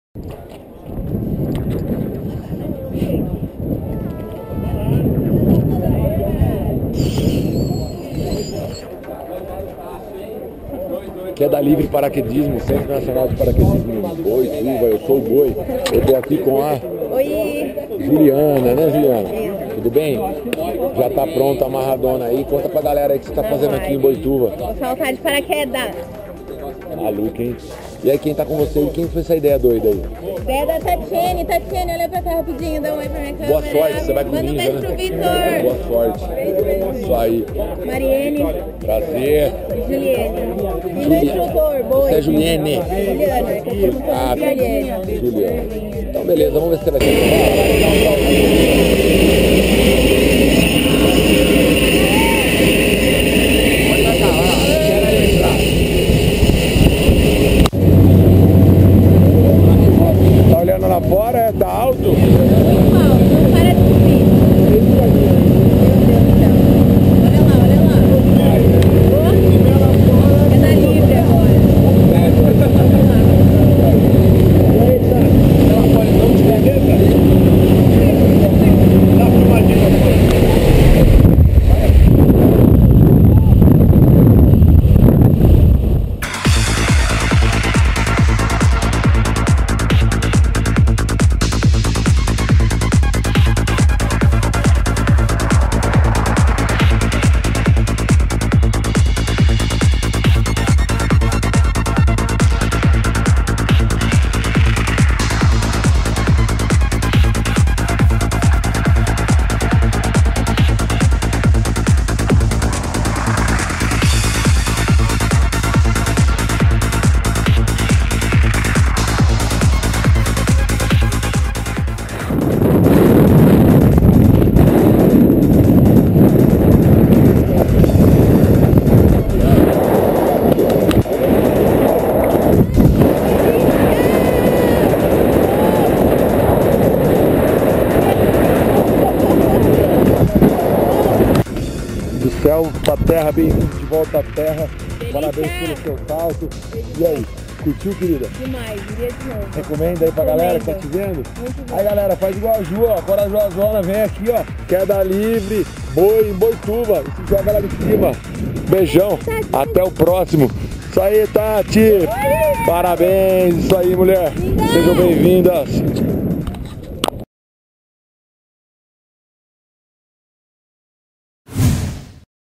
Salto de Paraquedas da Juliana na Queda Livre Paraquedismo
Data: 15/07/2012

Passageiro: Juliana

Instrutor: Boi

Escola: Queda Livre Paraquedismo